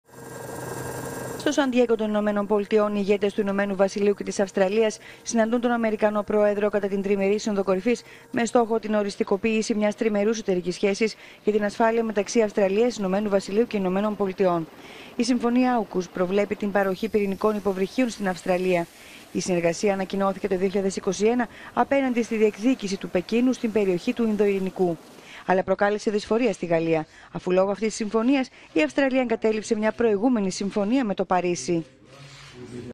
Οριστικοποιείται η AUKUS μεταξύ ΗΠΑ, Ηνωμένου Βασιλείου και Αυστραλίας
Κατά την τριμερή Σύνοδο Κορυφής στο Σαν Ντιέγκο